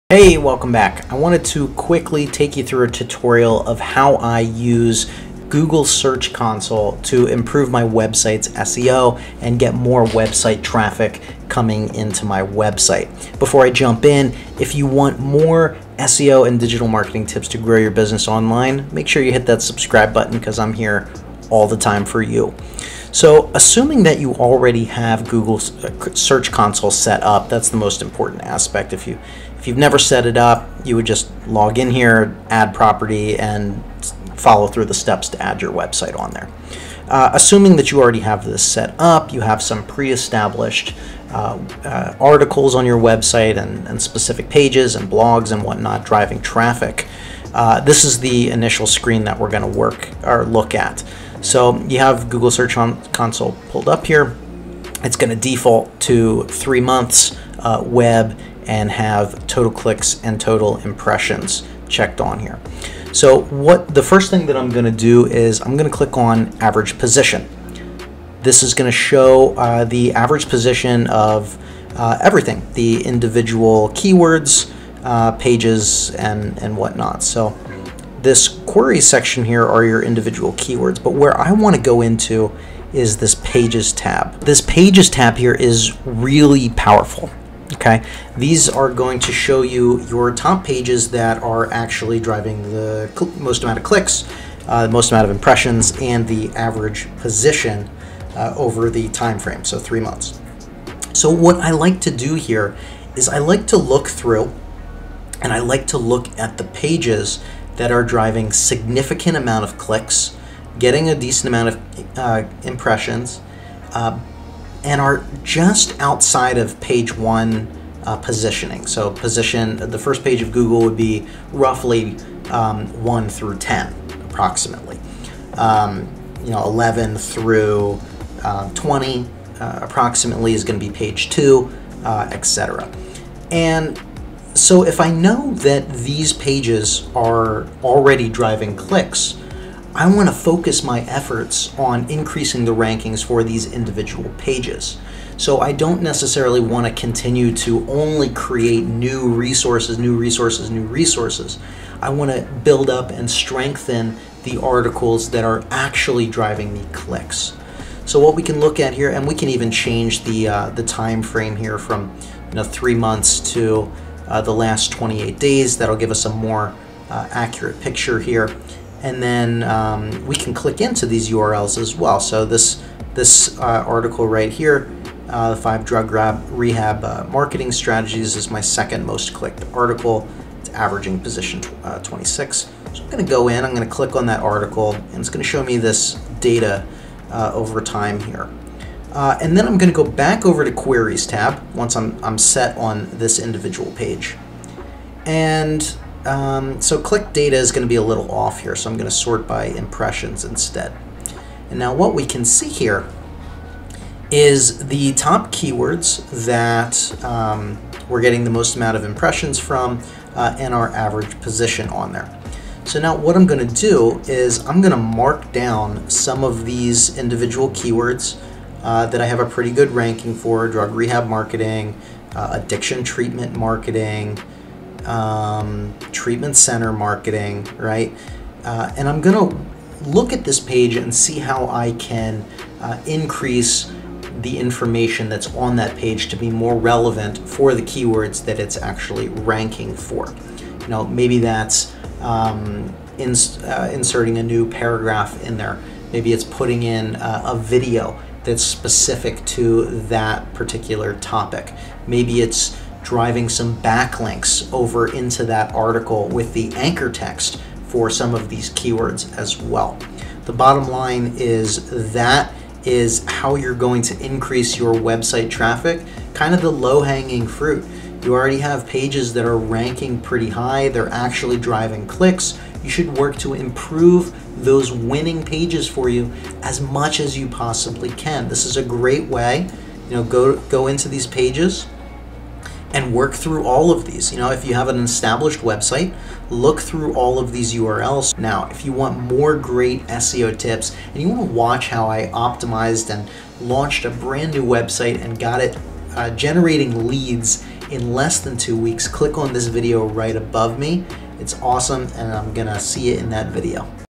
How You Can Increase Website Traffic W/ Google Search Console In 2021
Here's how I use Google Search Console to increase my website traffic in 2021. Follow along so you can too!

In 2021, SEO is still an essential way to increase your website traffic! Google Search Console is an amazing free tool to see what your top keywords and pages are that are actually driving traffic to your website! What's the secret here? Pay attention to your already winning pages, and make them better!